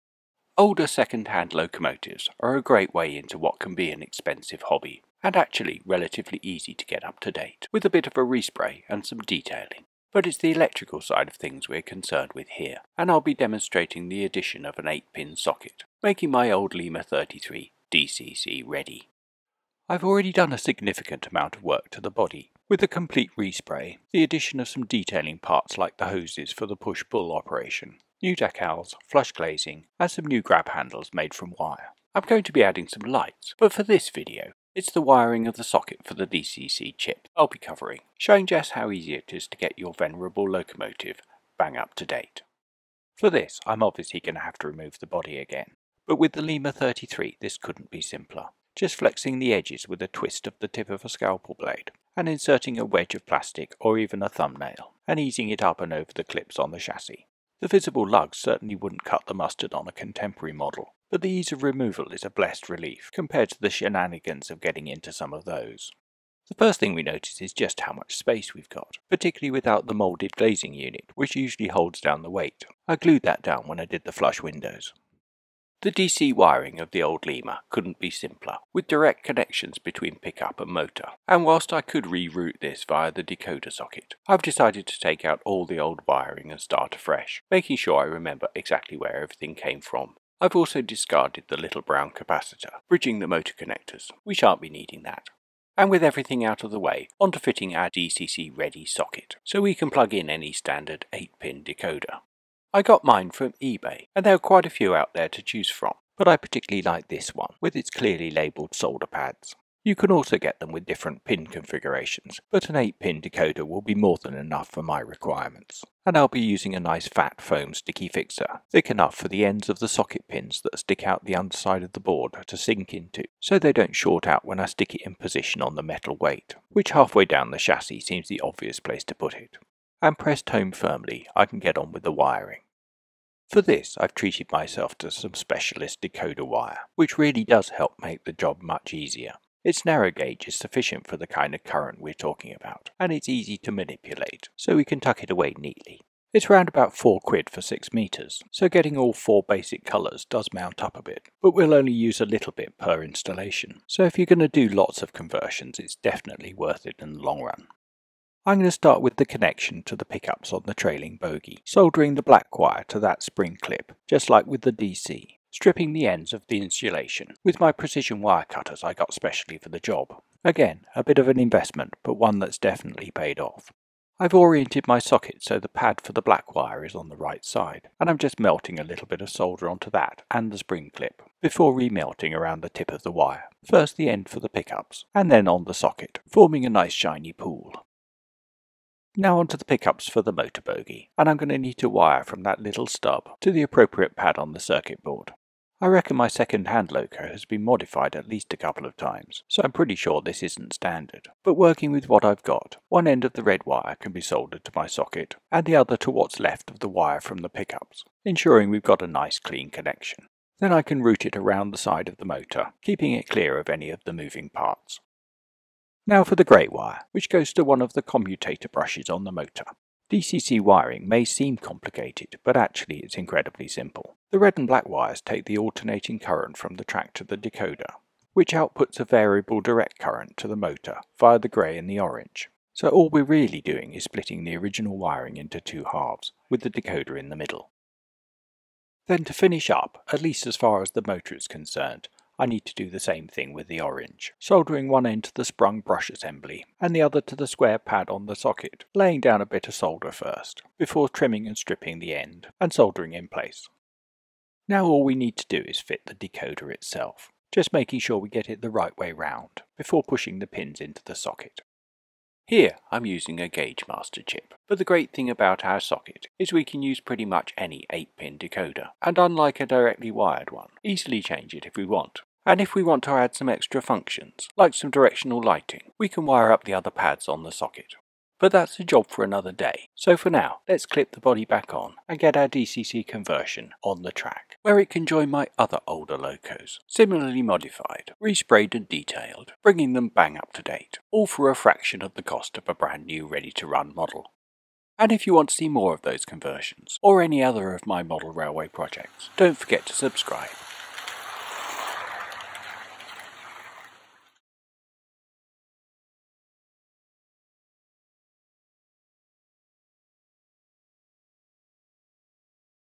Adding an 8-pin DCC decoder socket.to older locos
Older, second-hand locos are a cheap way of getting into model railways, but without a decoder socket they are far from “DCC-ready". Demonstrating with a vintage Lima Class 33, I show how easy it is to add one for yourself, with inexpensive parts and just basic soldering skills, bringing your loco bang up-to-date for the digital era.

For the socket, search "8 pin DCC socket" on eBay or other sites. Expect to pay about £1.50 plus P&P. All of the product endorsements are my own, and not paid.

Note, the video is based on an edited section of my full Class 33 lighting tutorial, the full version of which can be seen

Chapters:

0:00 Intro
0:19 My Lima Class 33
0:43 Body removal
1:06 Removing the old DC wiring
1:36 The new 8-pin DCC socket
2:13 Wiring the socket:
2:39 Soldering the black wire...
3:09 ...and the red
3:35 Adding the grey wire to the motor...
4:00 ...and the orange
4:18 Installing the decoder
4:38 Replacing the body
4:44 Running the converted loco